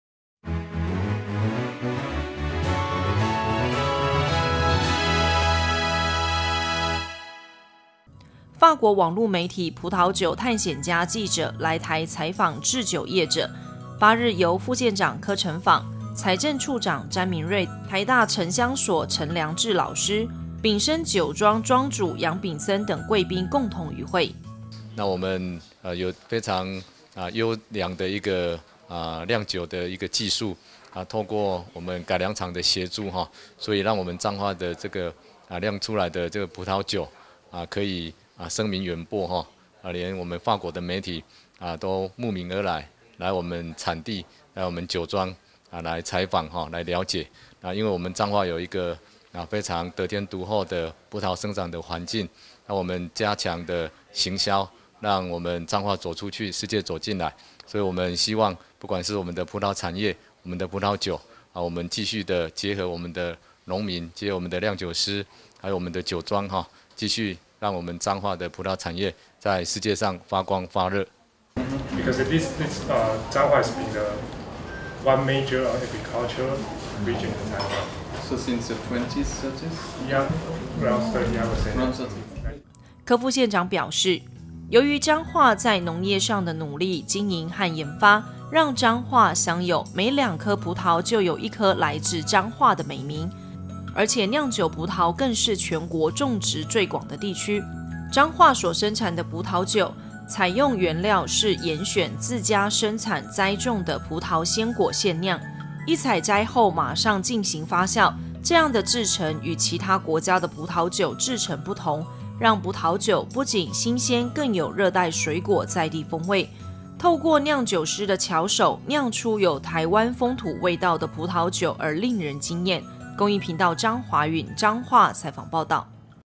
103-06-08 法國媒體訪彰縣，葡萄釀酒產業令人驚豔
法國網路媒體葡萄酒探險家(Wine Explorers)記者Jean-Baptiste與Ludovic Pollet來台採訪製酒業者，(8)日由副縣長柯呈枋，財政處長詹明叡等人在縣長室熱情接待。與會貴賓有台大城鄉所陳良治老師、秉森酒莊莊主楊秉森等人！柯副縣長表示，由於彰化在農業上的努力經營和研發，讓彰化享有「每兩顆葡萄就有一顆來自於彰化」的美名，而且釀酒葡萄更是全國種植最廣的地區，彰化所生產的葡萄酒採用原料是嚴選自家生產栽種的葡萄鮮果現釀，一採摘後馬上進行發酵，這樣的製程與其他國家的葡萄酒製程不同，讓葡萄酒不僅新鮮更有熱帶水果在地風味，像秉森酒莊就利用黑后葡萄，透過釀酒師的巧手釀出有台灣風土味道的葡萄酒而令人驚豔。